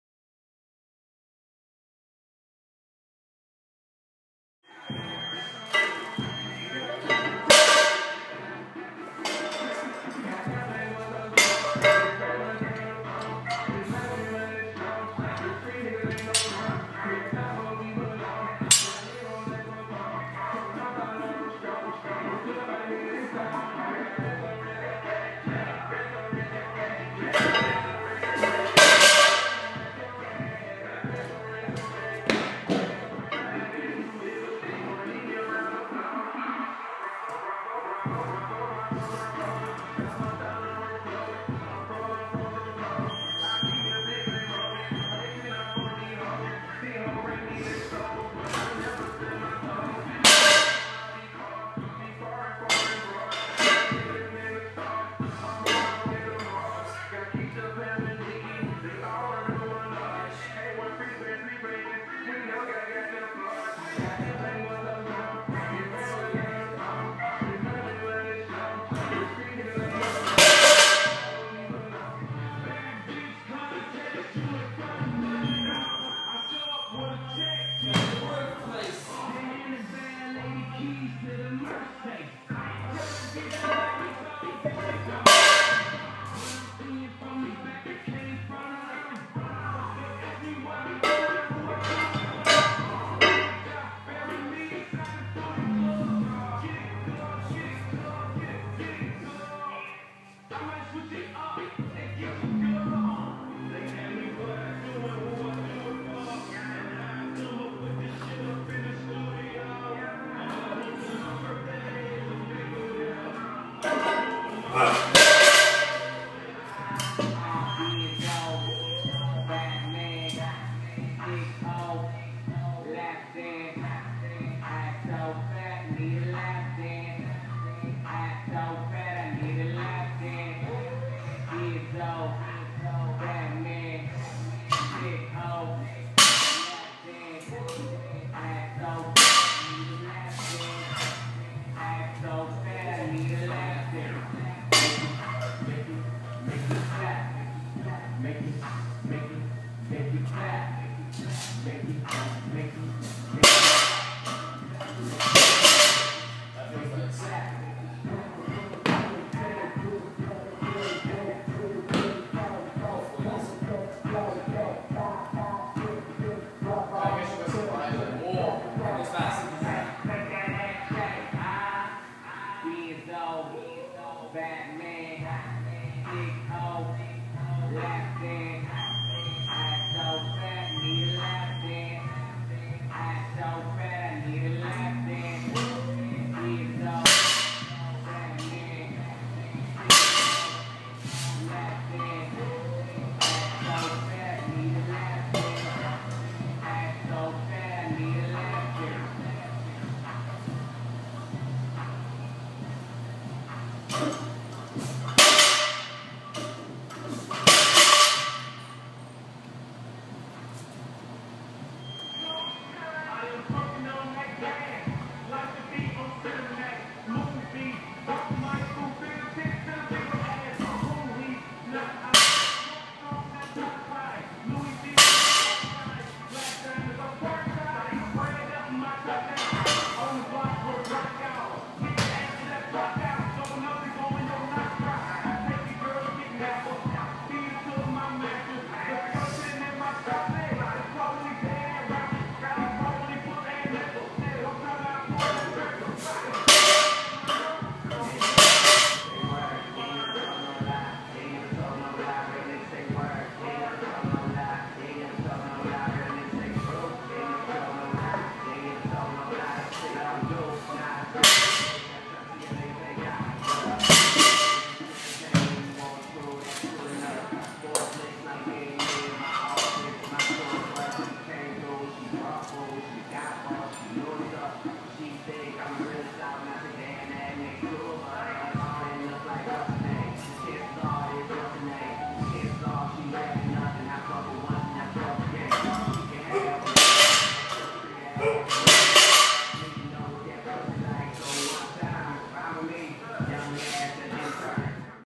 04.01.13 Training
Brett Lewandowsky and Willie Albert performing banded deadlifts for speed at Crossfit Otown